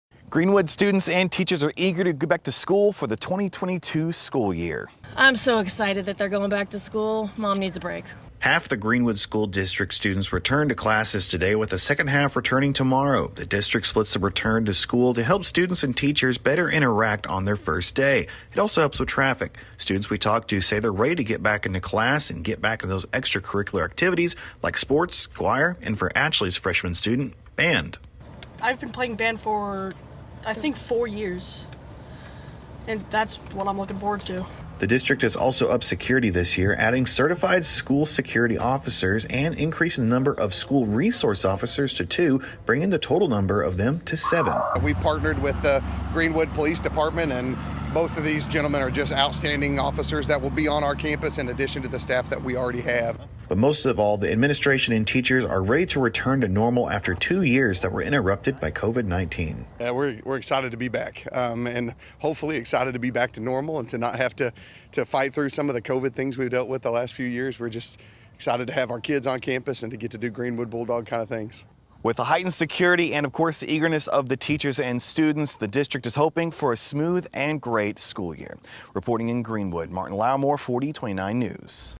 Greenwood students head back to school
Students went back to school in Greenwood on Monday.